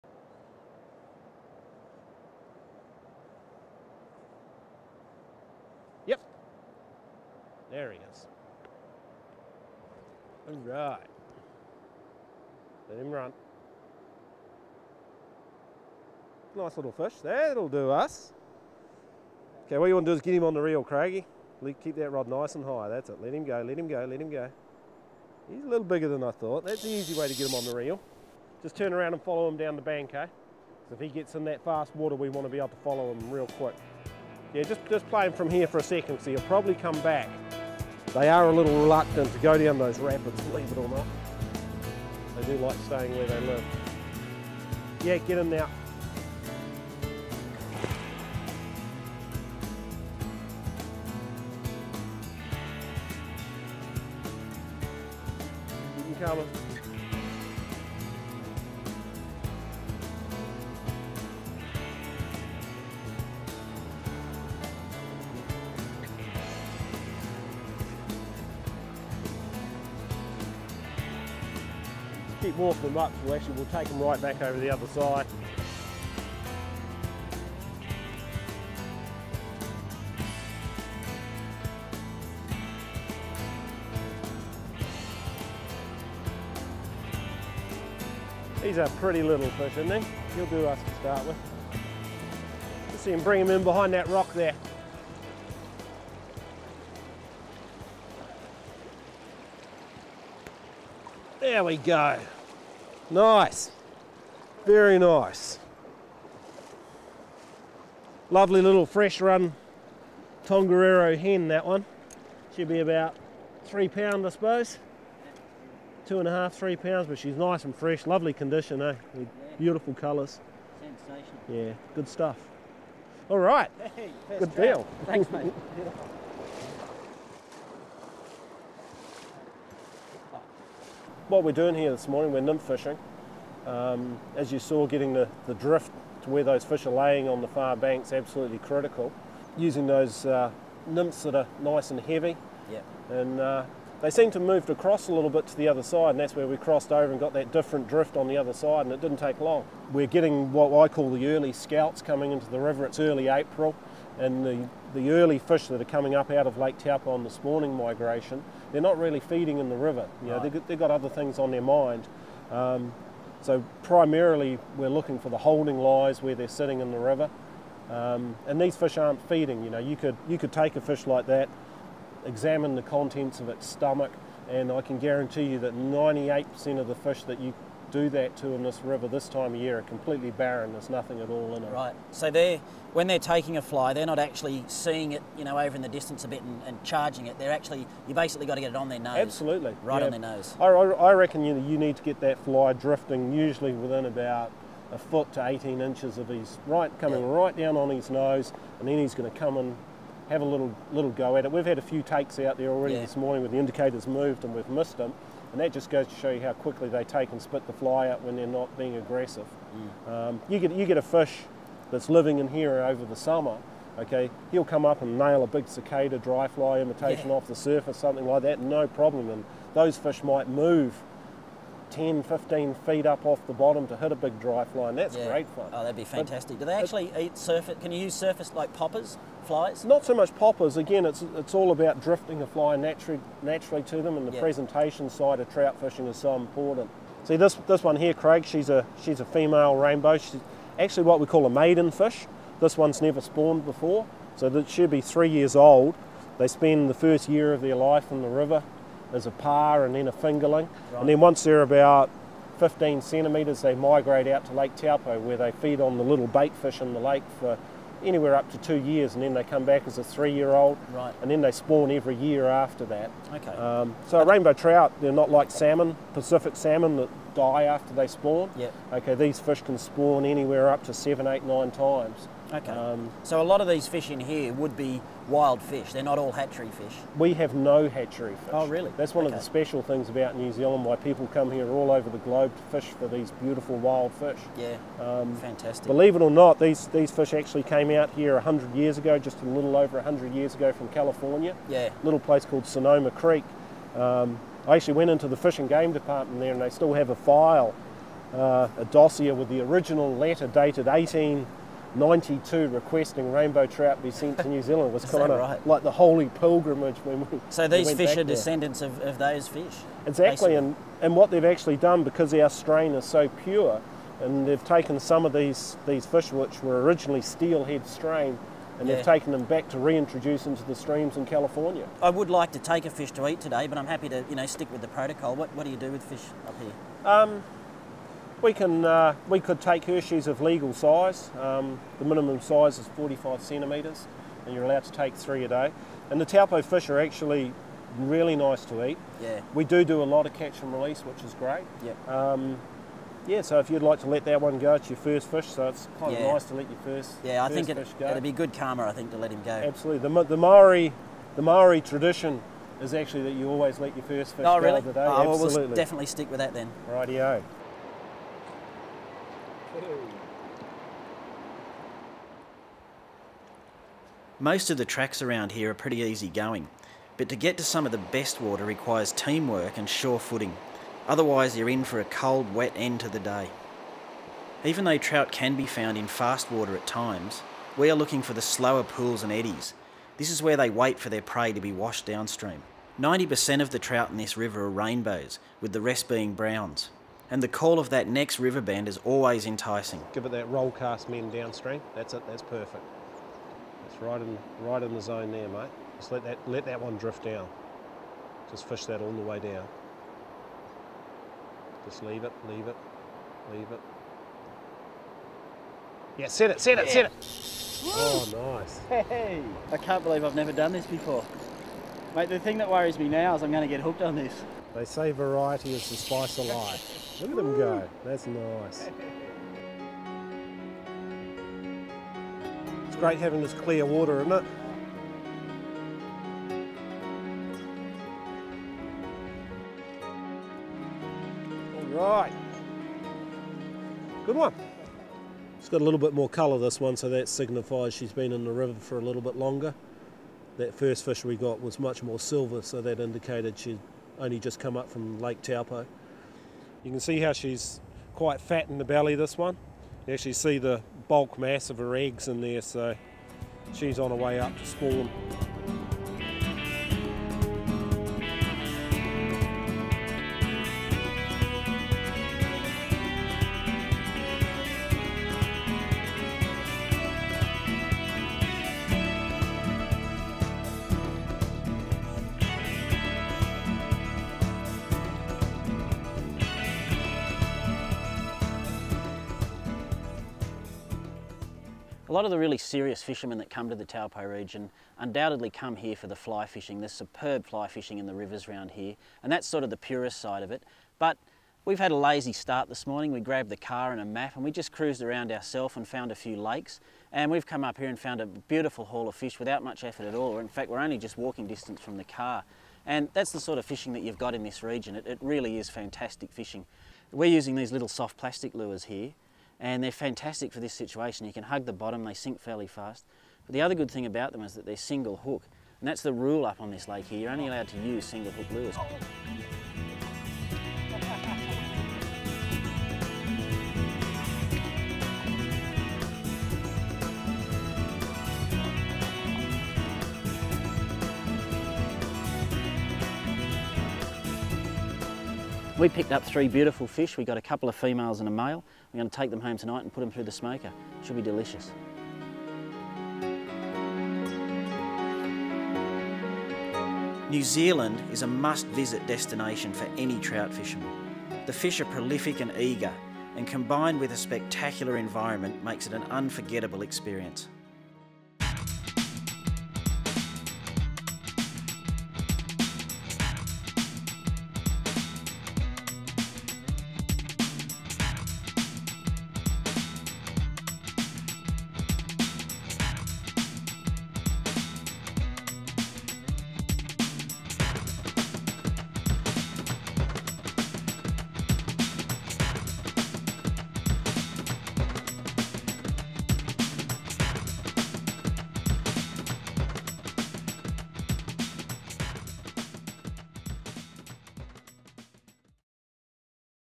Trout Fishing in the Tongariro River, New Zealand; Part 2 of 2
Learn how to catch Trout on flys and lures with some of the best fishing guides in the world. Discover just how good the trout fishing is in the Tongariro River, New Zealand.